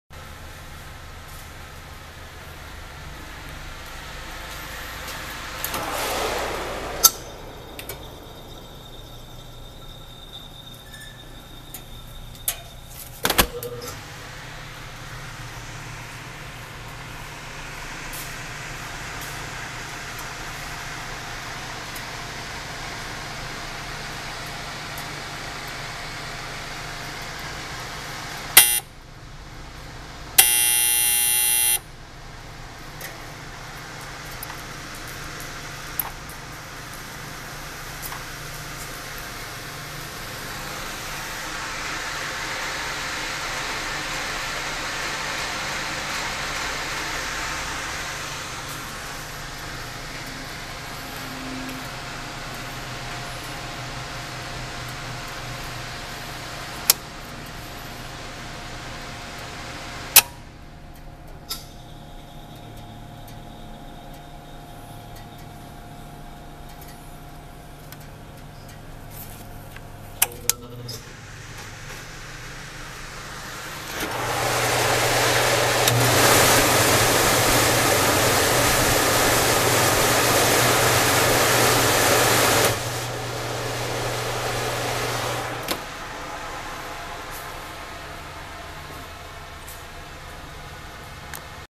Blodgett Mark V cook n hold
This Mark V convection oven is also a cook n hold.
It came from the school board. Can be single or three phase.